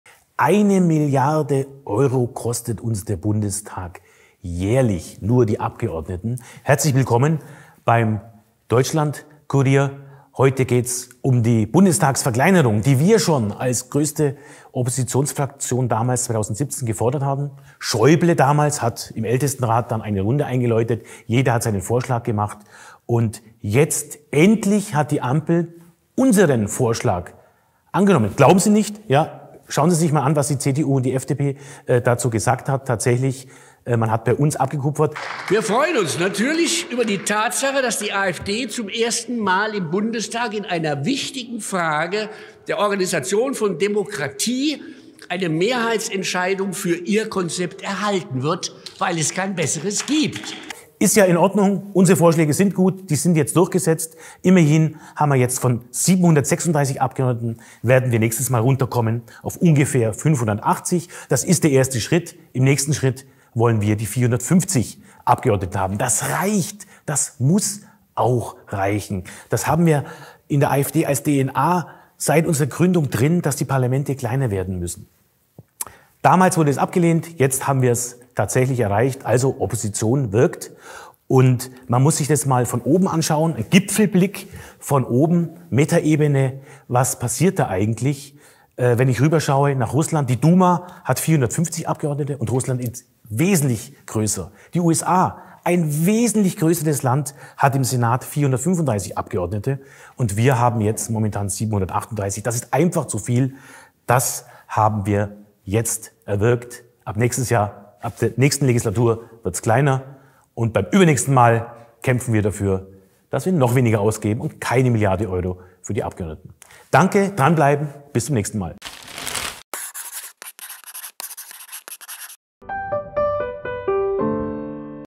„Felsers Gipfelblick“: Verschlankung des Parlaments geplant - AfD wirkt! | Peter Felser (AfD)
Eine Milliarde Euro Kosten für den den Bundestag jährlich - und das allein nur für die Abgeordneten!
Jetzt hat die „Ampel“ den Vorschlag der AfD übernommen und strebt eine Verschlankung des Parlaments an.

Ein Kommentar des DeutschlandKURIER🇩🇪-Kolumnisten Peter Felser, bayerischer AfD-Bundestagsabgeordneter.